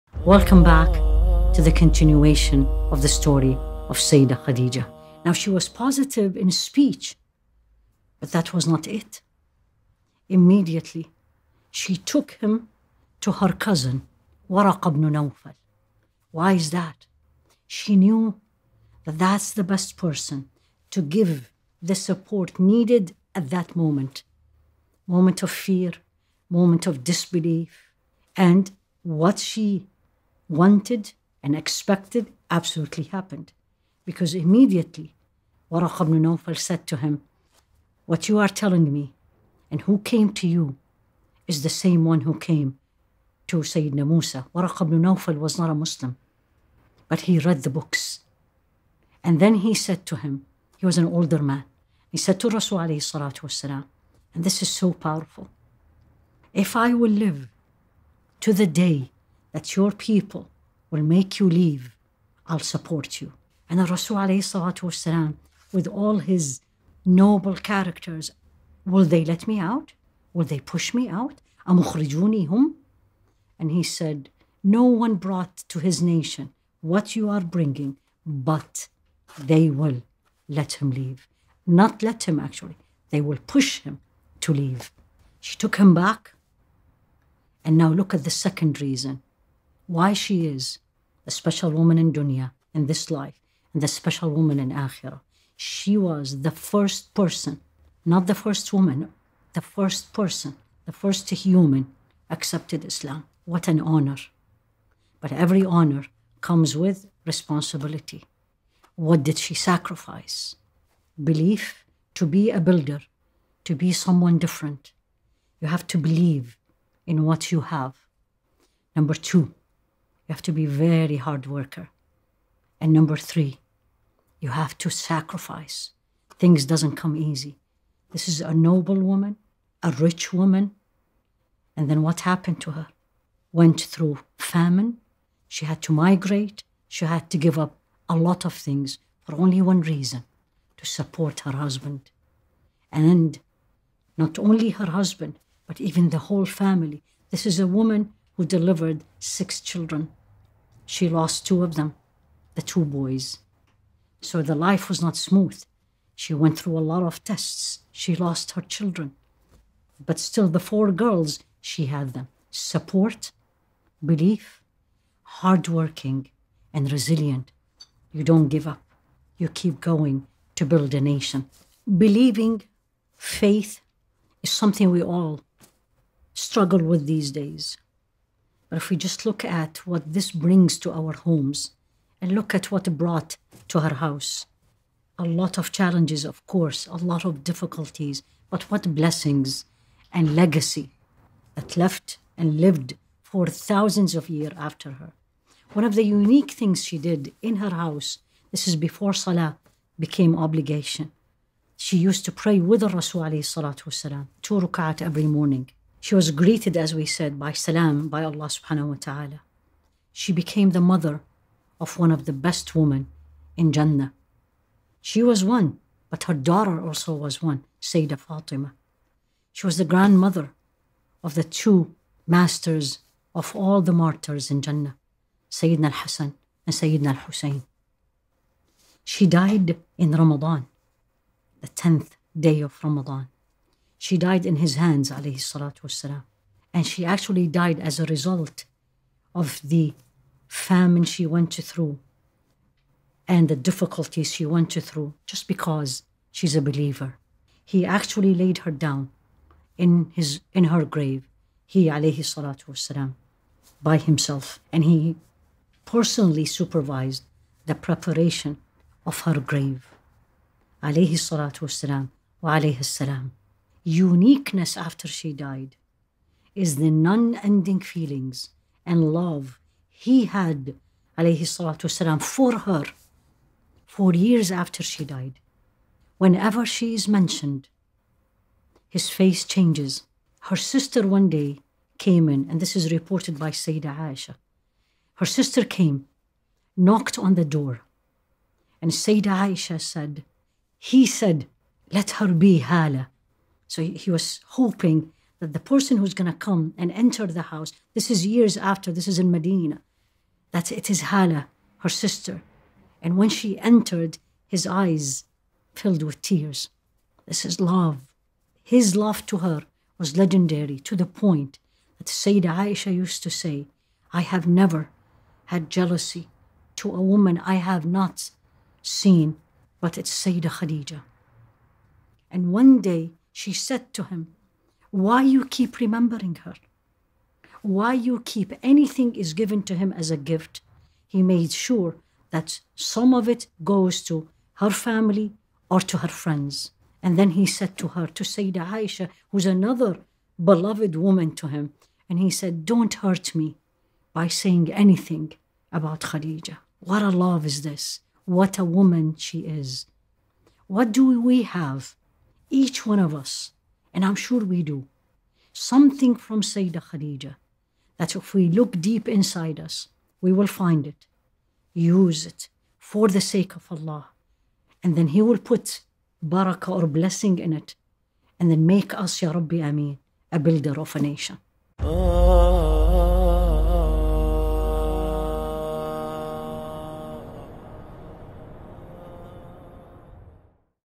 Khadijah bint Khuwaylid (ra) ｜ Part II ｜ Builders of a Nation ｜ Dr Haifaa Younis ｜ Jannah Institute
Allah's Message
Islam = Peace

Lets Just Spread Peace in the World.